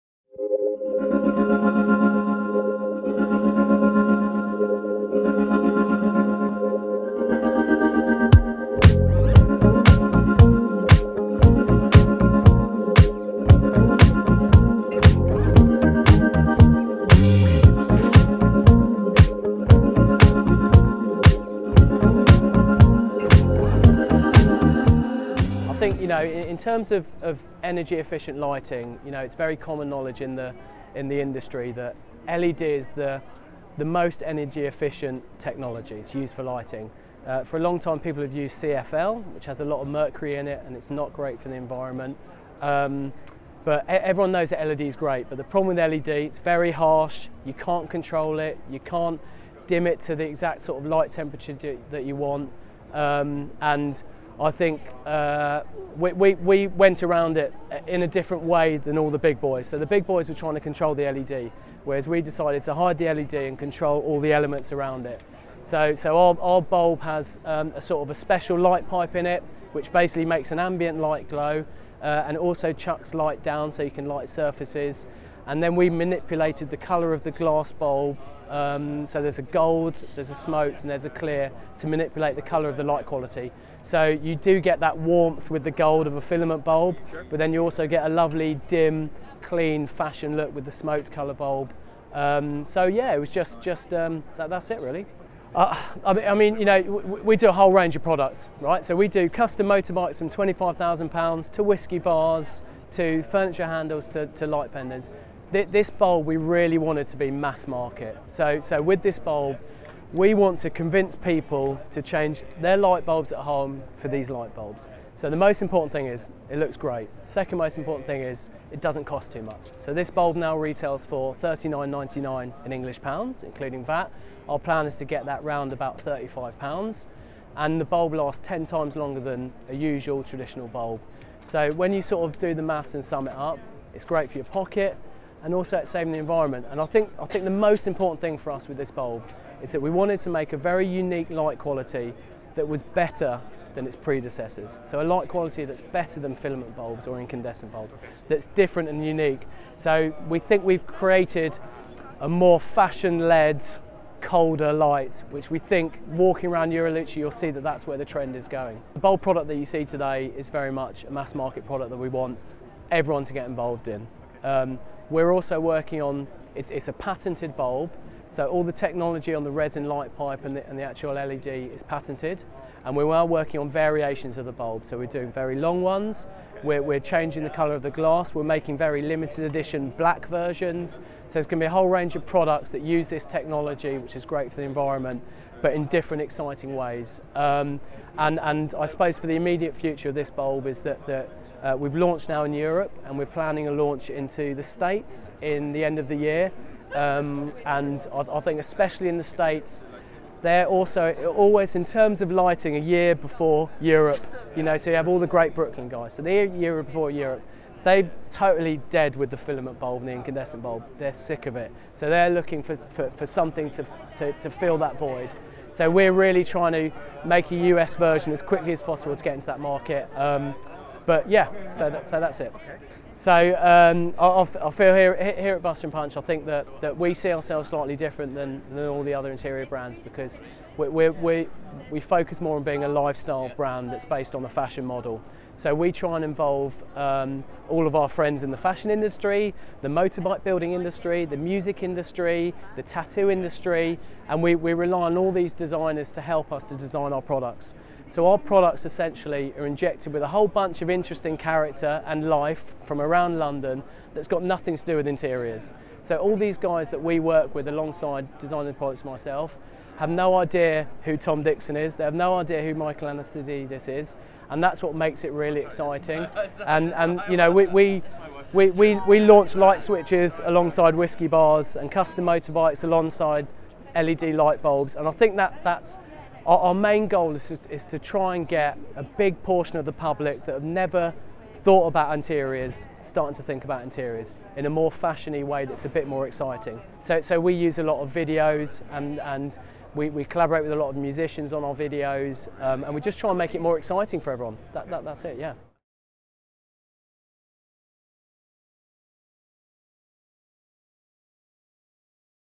Differentiating from the LED Big Boys
Starting life in a small garage in East London by making one-off custom motorcycles, British brand Buster + Punch launched light fittings and furniture in 2012. Much of the work by Buster + Punch is based around precious metals, rubber and lighting, and the brand has a large degree of “rock ‘n’ roll” in its DNA.
Fast forward to the present day and Buster + Punch has presented the Buster Bulb, the world’s first superlatively designed LED bulb. Founder and designer Massimo Buster Minale explains more about the brand and its products in this interview during iSaloni 2015.

Flip back to our live coverage of iSaloni to read more about the brand’s presentation during the fair

Courtesy of ArchiExpo